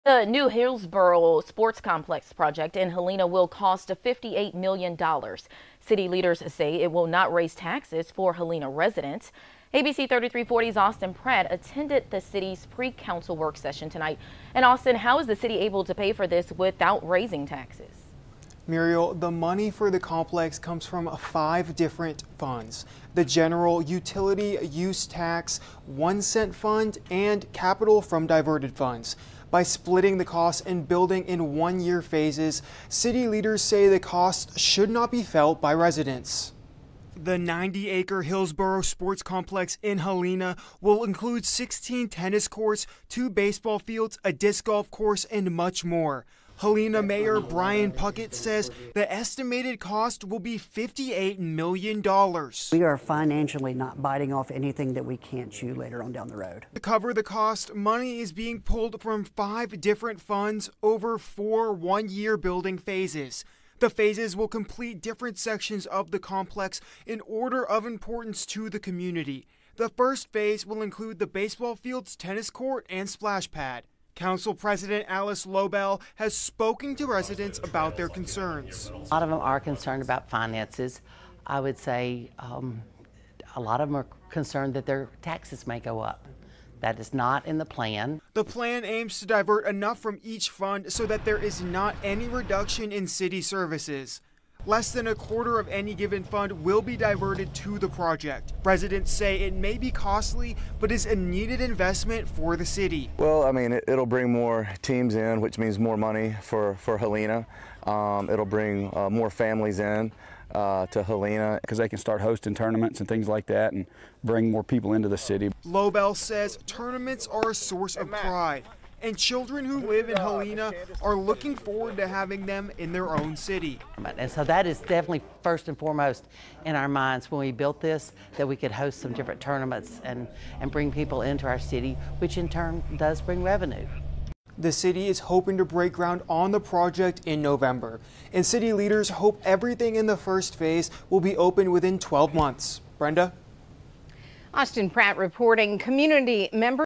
Cost of new Helena sports complex revealed
The Hillsboro Sports Complex is a 90 acre facility including many new baseball fields, tennis courts, a disc golf course and so much more coming to Helena. It is estimated to cost $85 million -- MORE